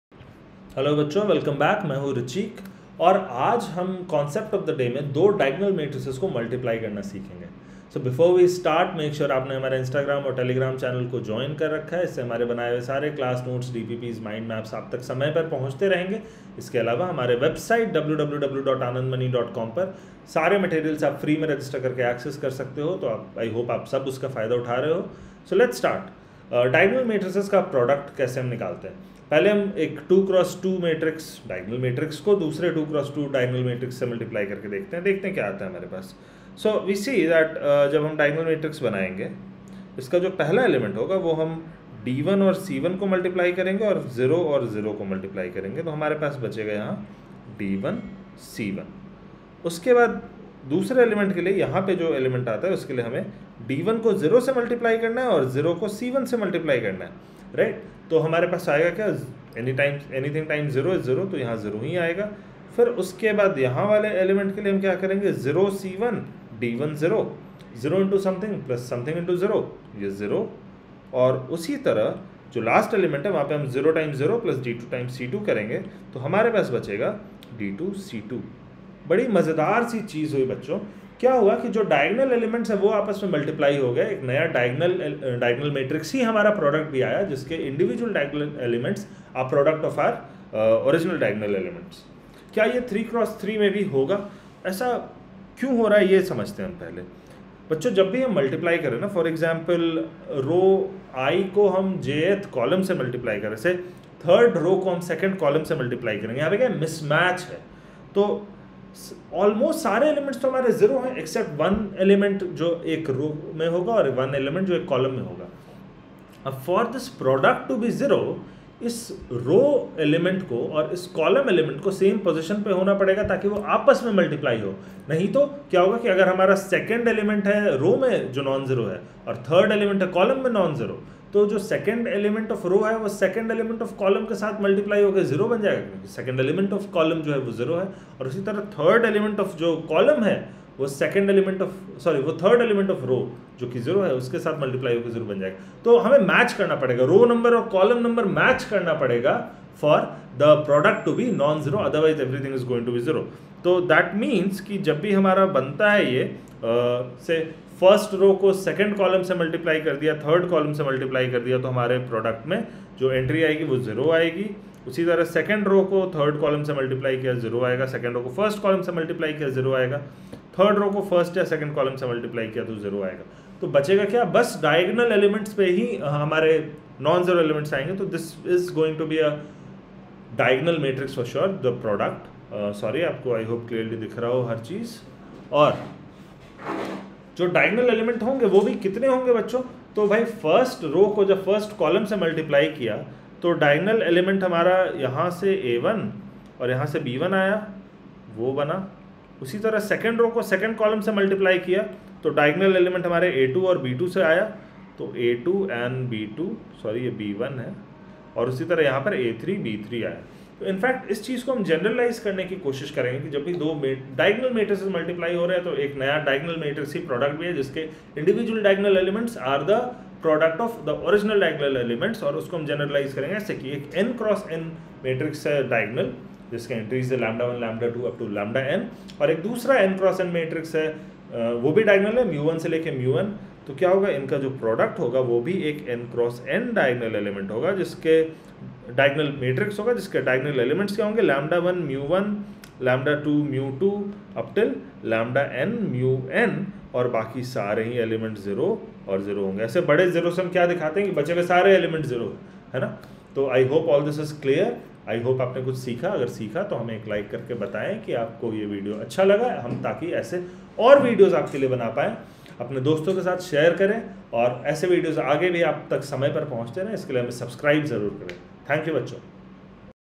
Multiplying Diagonal Matrices | Concept of the day | Dr.Anand Mani and Team
In this video Richick Sir will be discussing the concept of Multiplying Diagonal Matrices: Arithmetic Progression. He is explaining the whole process of how to calculate diagonal matrices product.